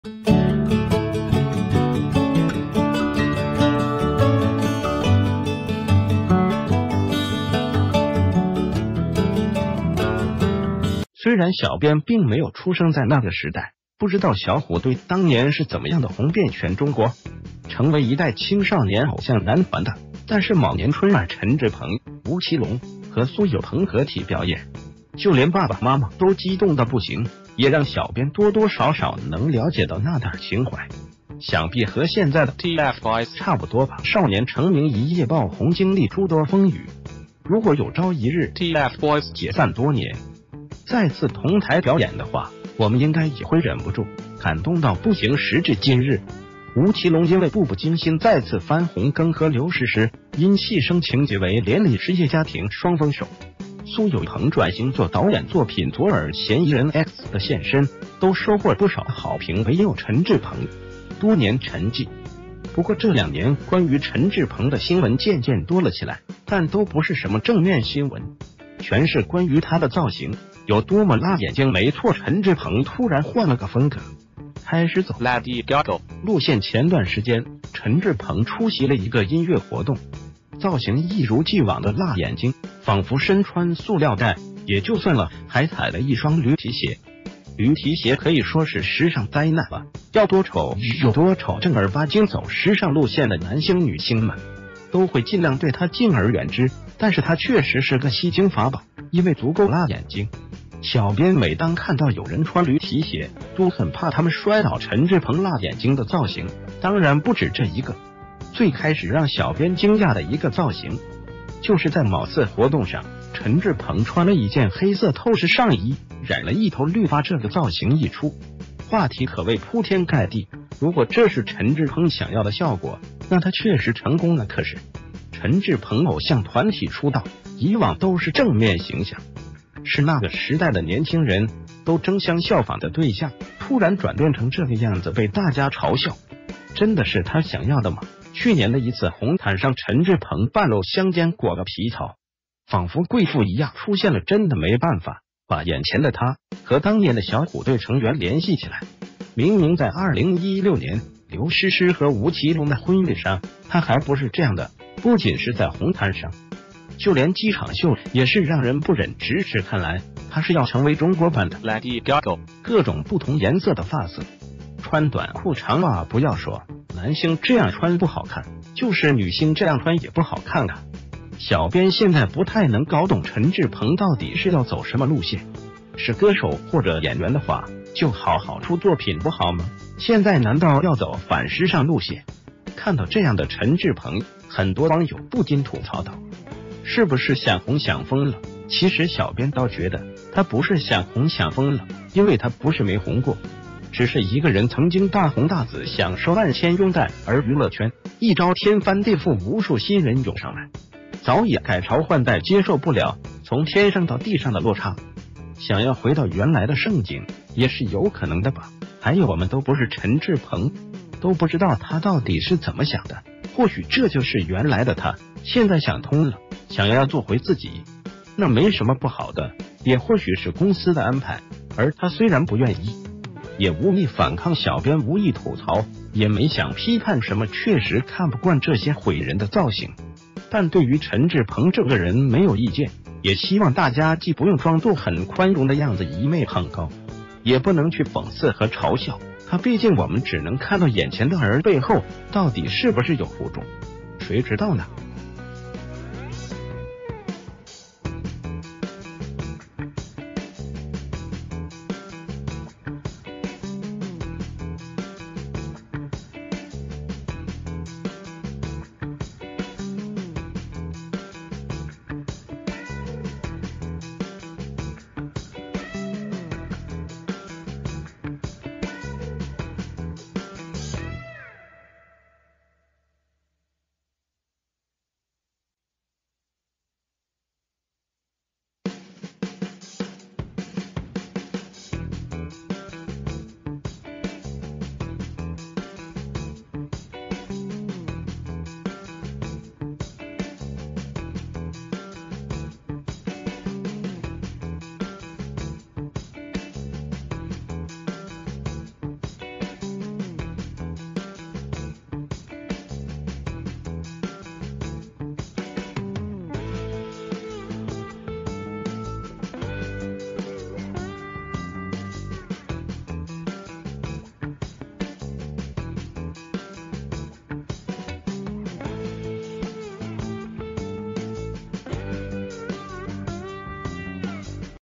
陳志朋被稱為中國版LadyGaga？造型辣眼睛，想紅想瘋了！
谢谢！

    雖然小編并沒有出生在那個時代，不知道小虎隊當年是怎么樣的紅遍全中國，成為一代青少年偶像男團的。但是某年春晚陳志朋、吳奇隆和蘇有朋合體表演，就連爸爸媽媽都激動到不行，也讓小編多多少少能了解到那點兒情懷。想必和現在的TFBOYS差不多吧，少年成名一夜爆紅，經歷諸多風雨。如果有朝一日TFBOYS解散多年，再次同臺表演的話，我們應該也會忍不住感動到不行。時至今日，吳奇隆因為《步步驚心》再次翻紅，更和劉詩詩因戲生情結為連理，事業家庭雙豐收。蘇有朋轉型做導演，作品《左耳》《嫌疑人X的獻身》都收獲不少好評。唯有陳志朋，多年沉寂。
    不過這兩年關于陳志朋的新聞漸漸多了起來，但都不是什么正面新聞，全是關于他的造型有多么辣眼睛。沒錯，陳志朋突然換了個風格，開始走“LadyGaga”路線。前段時間陳志朋出席了一個音樂活動，造型一如既往地辣眼睛。仿佛身穿塑料袋也就算了，還踩了一雙驢蹄鞋。驢蹄鞋可以說是時尚災難了，要多丑有多丑。正兒八經走時尚路線的男星女星們，都會盡量對它敬而遠之。但是它確實是個吸睛法寶，因為足夠辣眼睛。小編每當看到有人穿驢蹄鞋，都很怕他們摔倒。
    陳志朋辣眼睛的造型當然不止這一個，最開始讓小編驚訝的一個造型，就是在某次活動上，陳志朋穿了一件黑色透視上衣，染了一頭綠發！這個造型一出，話題可謂鋪天蓋地。如果這是陳志朋想要的效果，那他確實成功了。可是，陳志朋偶像團體出道，以往都是正面形象，是那個時代的年輕人都爭相效仿的對象，突然轉變成這個樣子被大家嘲笑，真的是他想要的嗎？
    去年的一次紅毯上，陳志朋半露香肩裹個皮草，仿佛貴婦一樣出現了。真的沒辦法把眼前的他和當年的小虎隊成員聯系起來，明明在2016年劉詩詩和吳奇隆的婚禮上，他還不是這樣的！
    不僅是在紅毯上，就連機場秀也是讓人不忍直視，看來他誓要成為中國版的LadyGaga.各種不同顏色的發色，穿短褲長襪，不要說男星這樣穿不好看，就是女星這樣穿也不好看啊！小編現在不太能搞懂陳志朋到底是要走什么路線，是歌手或者演員的話就好好出作品不好嗎？現在難道要走反時尚路線？
    看到這樣的陳志朋，很多網友不禁吐槽道：“是不是想紅想瘋了？”其實小編倒覺得，他不是想紅想瘋了，因為他不是沒紅過。只是一個人曾經大紅大紫，享受萬千擁戴。而娛樂圈一朝天翻地覆，無數新人涌上來，早已改朝換代。接受不了從天上到地上的落差，想要回到原來的盛景，也是有可能的吧。
    還有，我們都不是陳志朋，都不知道他到底是怎么想的。或許這就是原來的他，現在想通了想要做回自己，那沒什么不好的。也或許是公司的安排，而他雖然不愿意，也無力反抗。小編無意吐槽，也沒想批判什么。確實看不慣這些毀人的造型，但對于陳志朋這個人沒有意見。也希望大家既不用裝作很寬容的樣子一昧捧高，也不能去諷刺和嘲笑他。畢竟我們只能看到眼前的，而背后到底是不是有苦衷，誰知道呢。